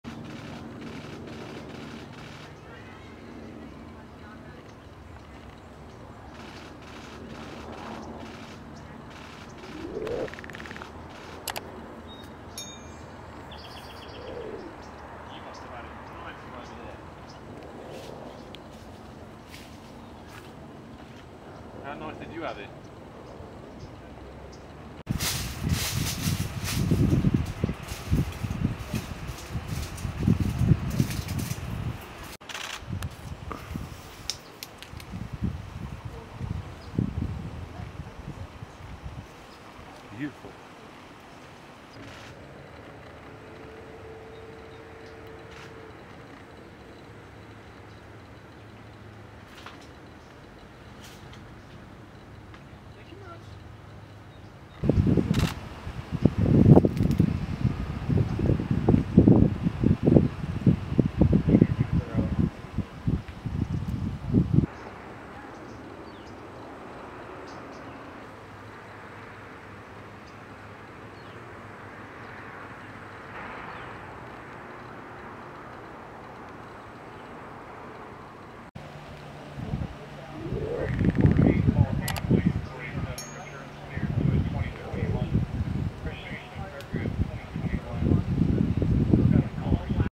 Kourtney Kardashian sets pulses racing in a risqué lingerie photoshoot in the Hollywood Hills
Kourtney Kardashian sets pulses racing in a risqué lingerie photoshoot in the Hollywood Hills. The reality star posed in various racy lingerie looks on Tuesday including one with a nipple ring attachment. Kourtney was seen in a black bustier and thong garter over wide fishnet stockings and sky high patent stilettos. In another look Kourtney’s black bra and mesh top showed a nipple ring attached to the outside. At another point in the day the recently engaged mom of three held on to her assistant Elizabeth as she balanced on another pair of white platform heels with a long silk nightgown and white robe on top. The 42 year old also showed off her perky posterior in a red corset thong. Happy Valentine’s Day Travis!